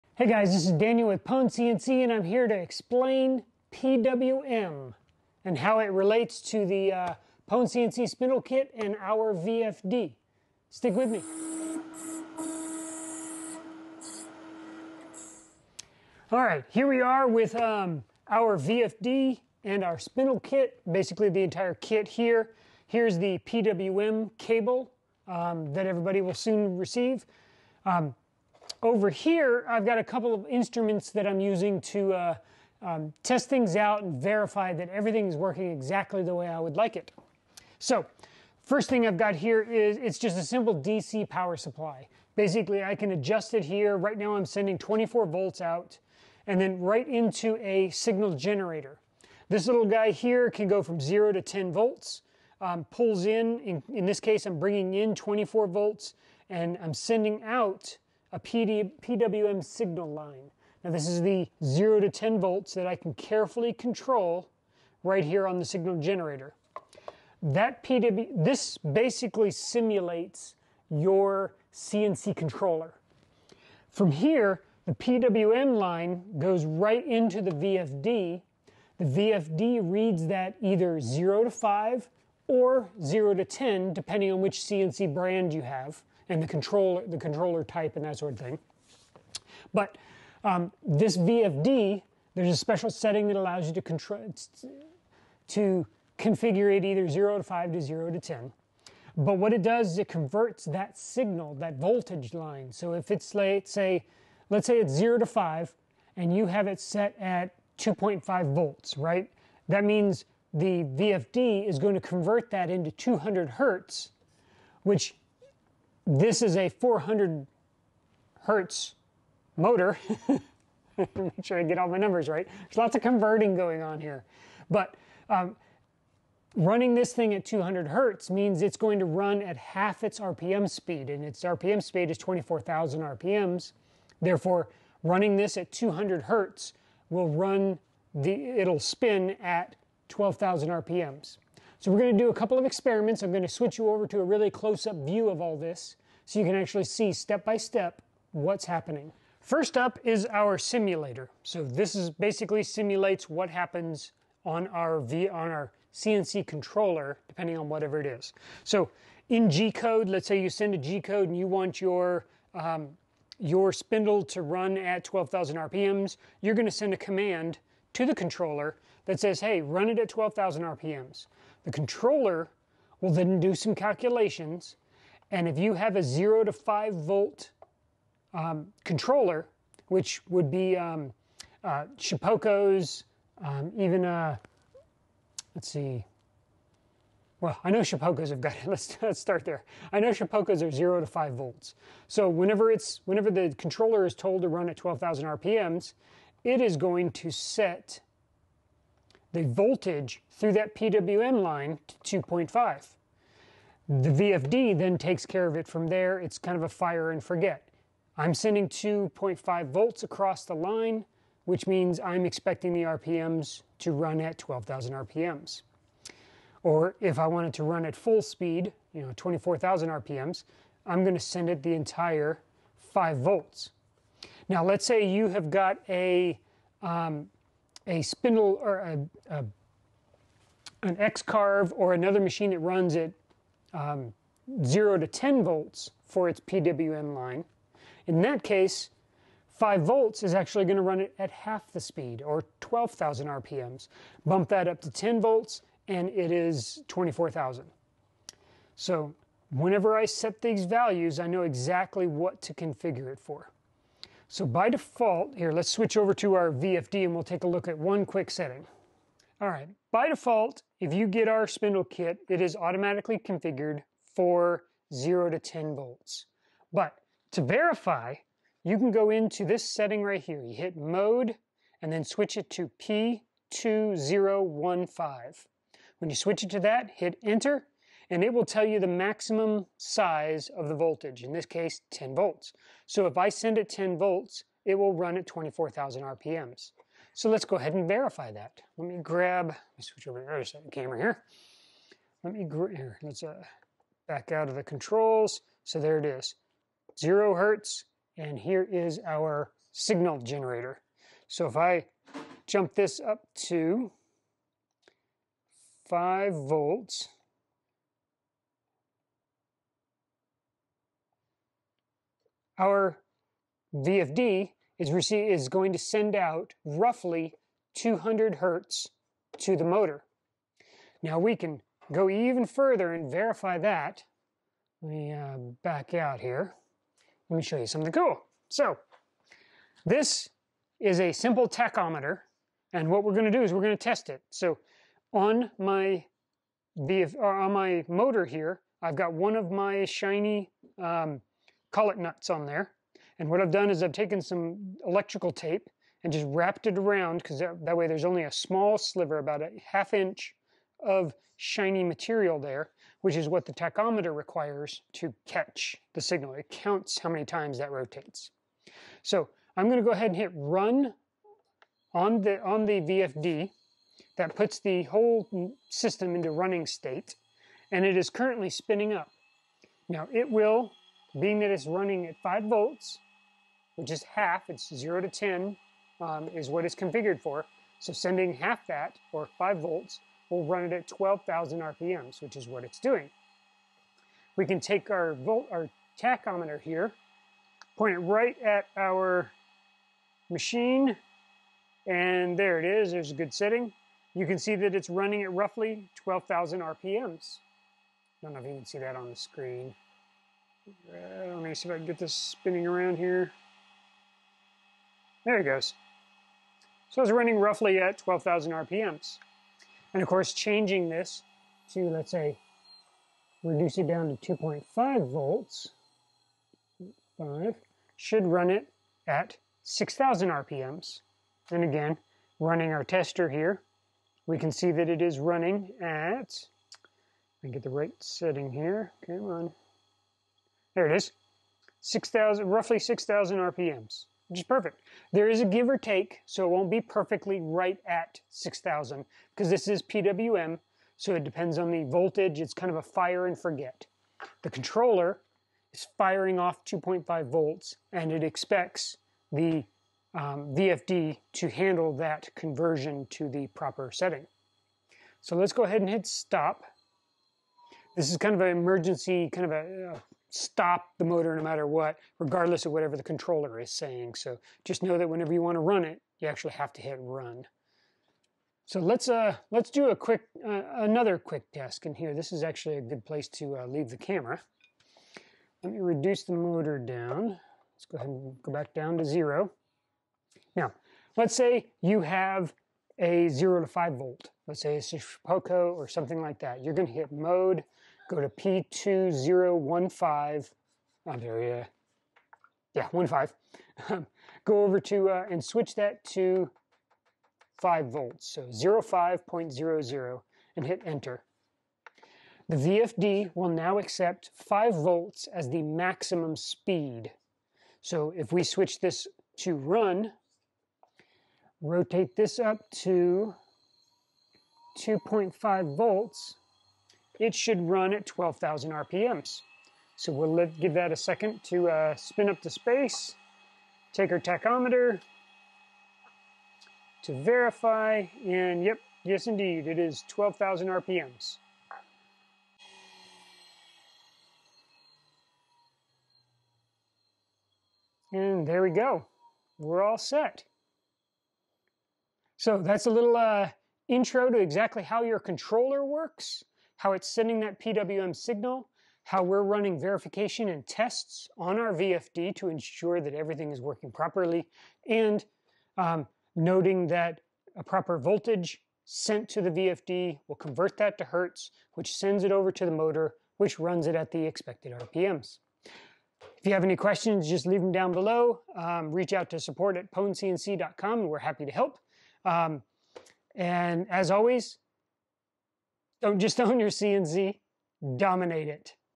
PwnCNC Spindle... PWM Testing
Note: Product features, bundles, and inclusions shown may differ from current offerings. Always refer to the product page for up-to-date information.

Have you ever wanted to know more about VFD's and how the PWM controls them?  This video covers the basics of PWM and I'll demonstrate how super easy it is to run tests.